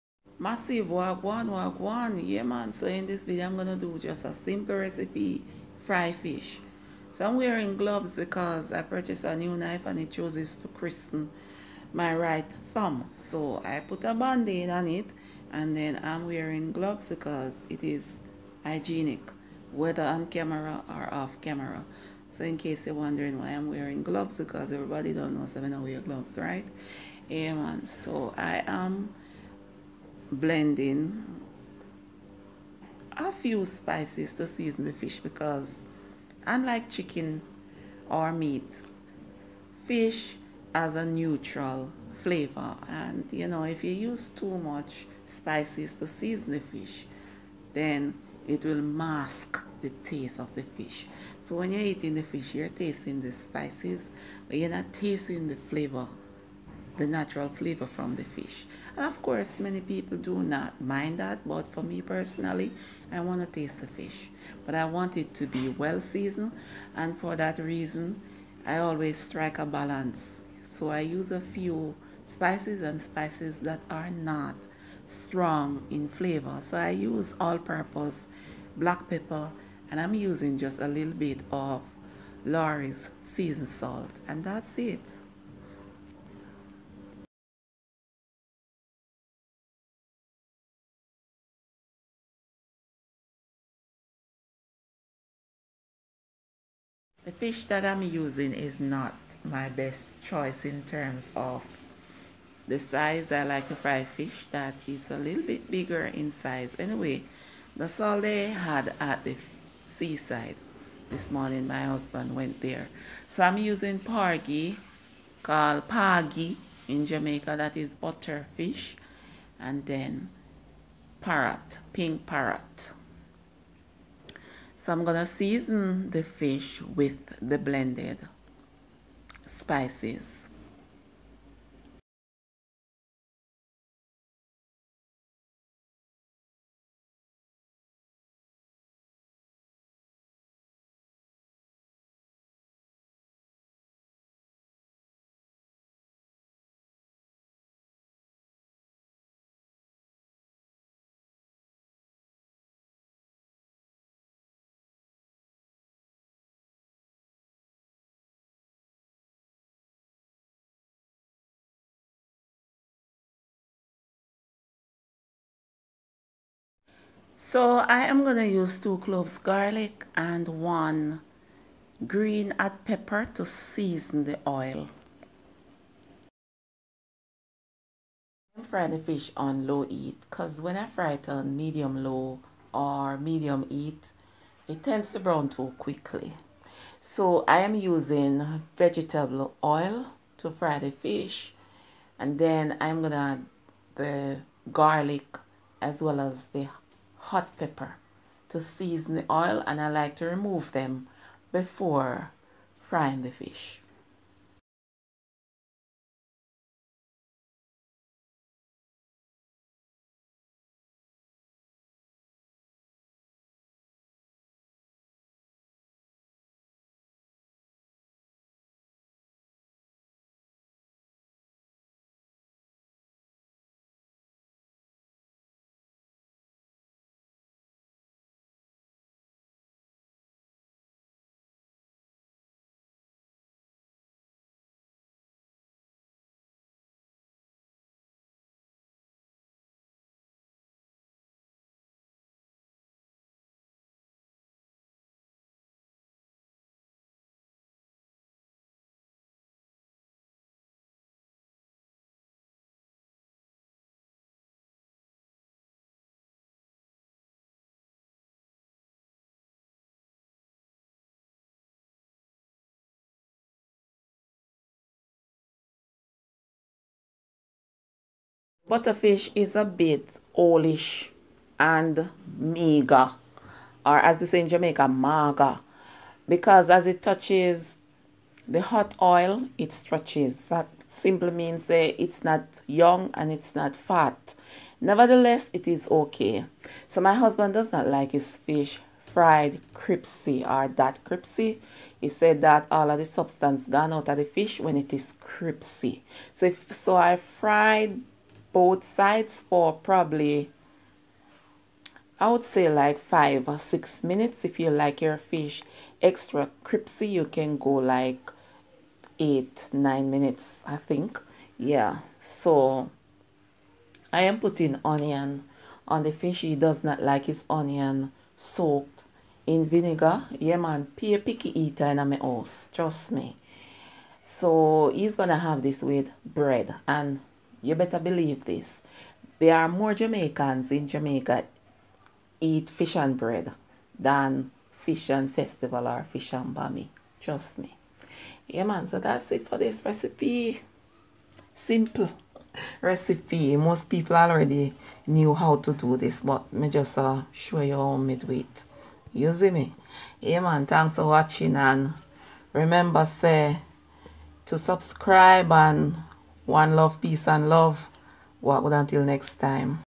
Fried Fish and Onion Jamaica style| Yard cooking Jamaica
This video is about fried fish, how to fry fish without flour